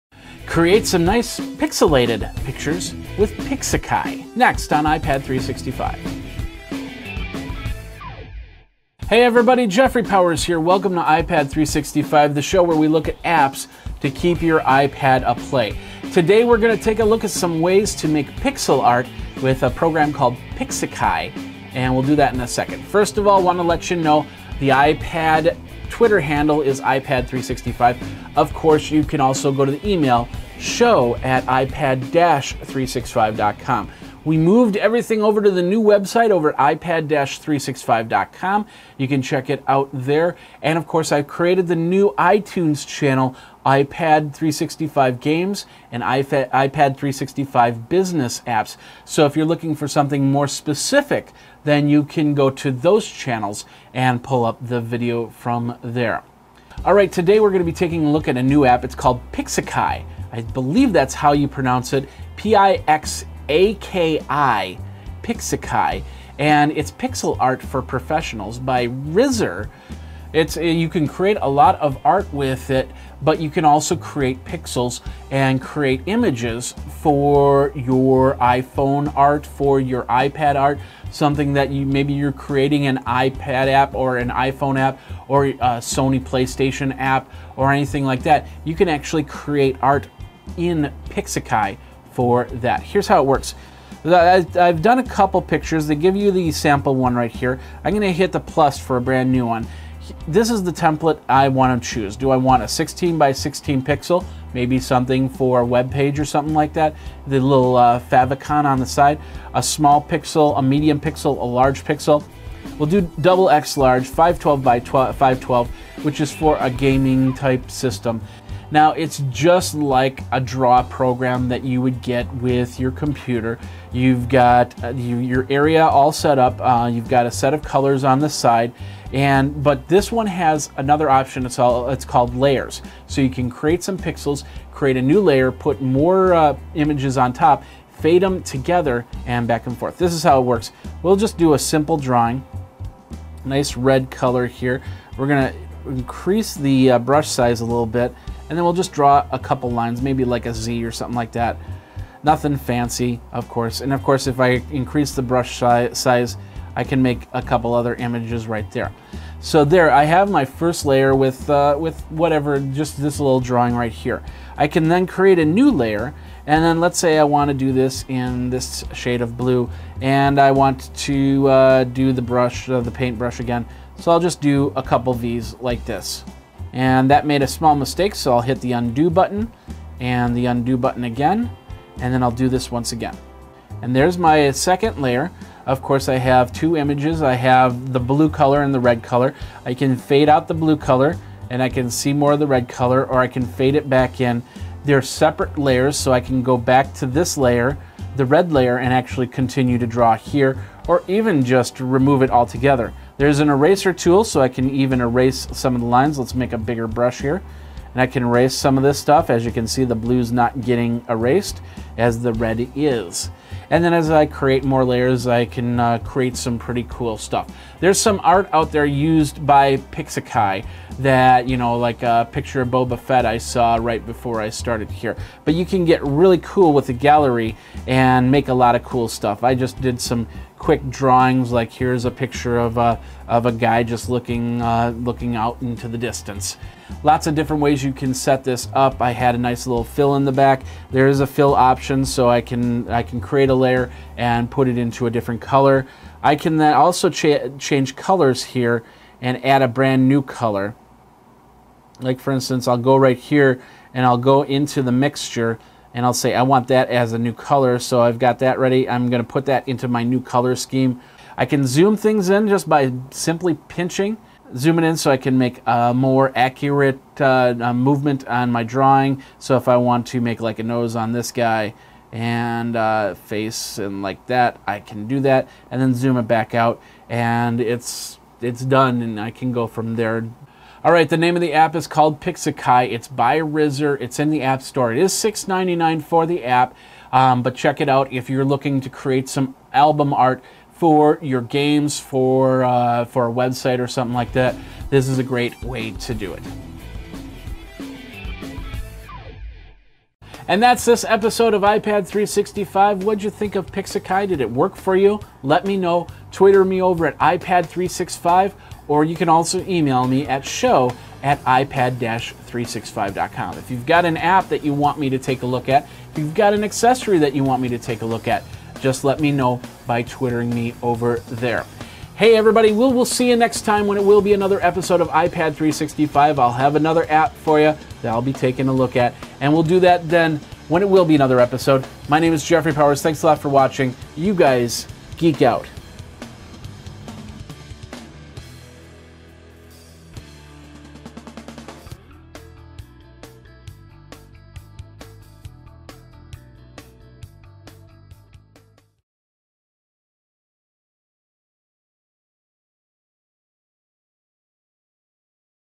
iPad365 Pixaki Pixel Drawing App for iPad
I look at Pixaki - a drawing app that helps create images, screens for games, apps and more.